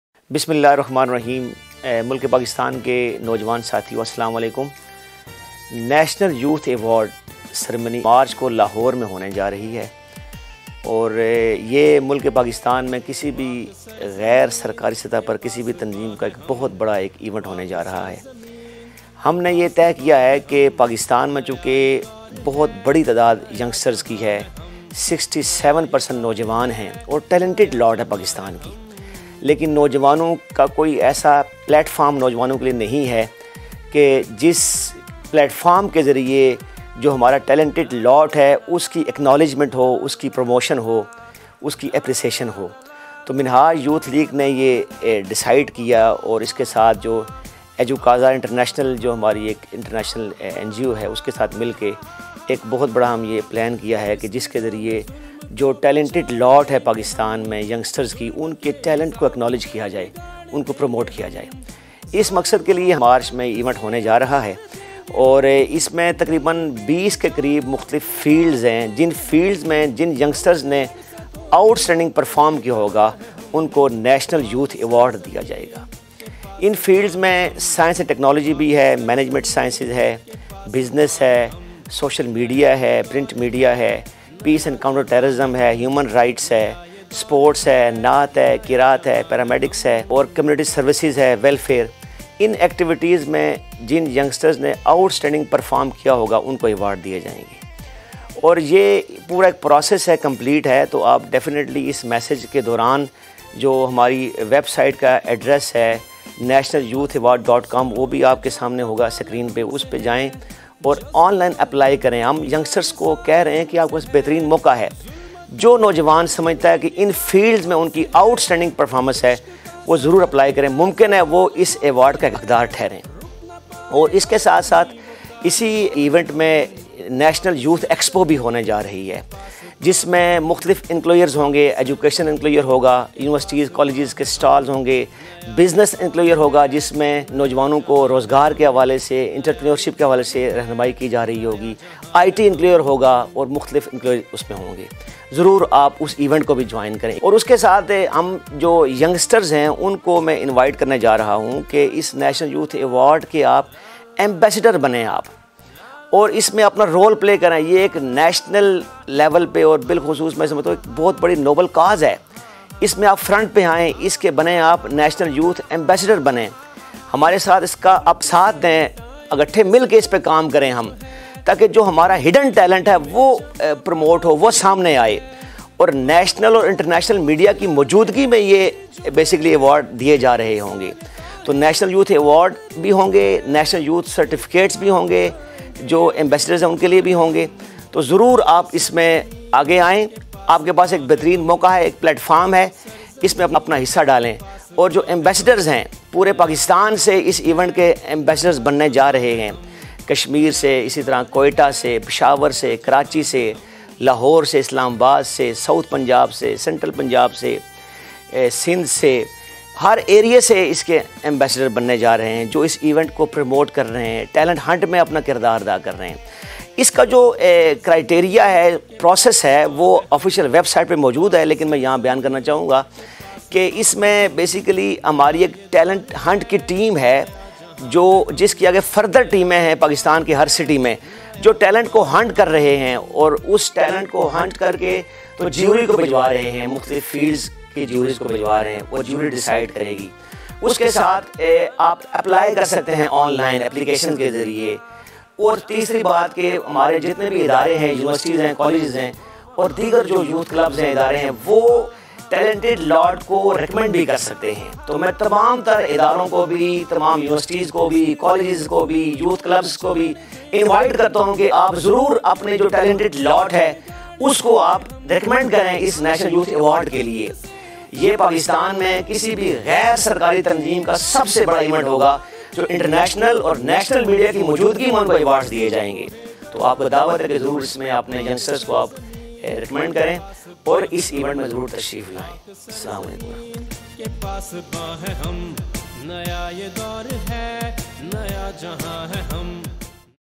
National Youth Award | National Youth Expo | 21-22 August at Jinnah Convention Center Islamabad
Apply Now...
Special Message for Youth Of Pakistan by Mazhar Mahmood Alvi Central President Minhaj Youth League Pakistan.
Apply Now.